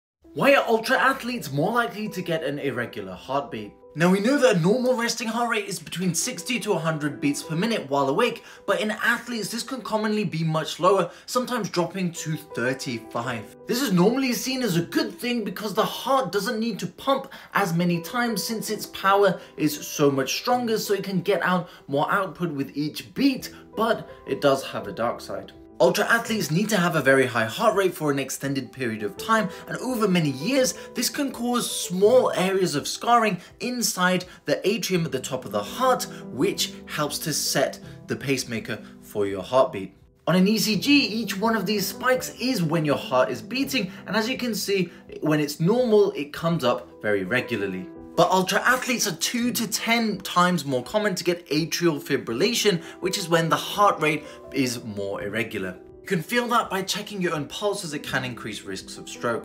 Why Ultra-Athletes Get Irregular Heartbeats
Why Ultra-Athletes Get Irregular Heartbeats pulse. Imagine your heart as a house with rooms. One of these rooms is called the atrium. Sometimes, after someone has heart surgery or a heart condition, scars can form around this atrium. These scars are like patches of rough terrain on a smooth road. Now, think of your heart's electrical system as the traffic lights that control the flow of cars on the road. When everything is working smoothly, the traffic lights signal in a regular pattern, allowing cars to move smoothly through intersections. But if there are bumps or potholes on the road, the traffic lights might not work properly. Similarly, when scars form around the atrium of the heart, they can disrupt the normal flow of electrical signals. These disruptions can cause irregular heartbeats, also known as arrhythmias. Instead of beating in a steady rhythm, the heart may beat too fast, too slow, or irregularly. This can make you feel like your heart is fluttering or pounding in your chest. While not all scars around the atrium lead to irregular heartbeats, some can interfere with the heart's electrical signals and cause problems. It's important for doctors to monitor and manage these conditions to help keep your heart beating smoothly and your blood flowing properly.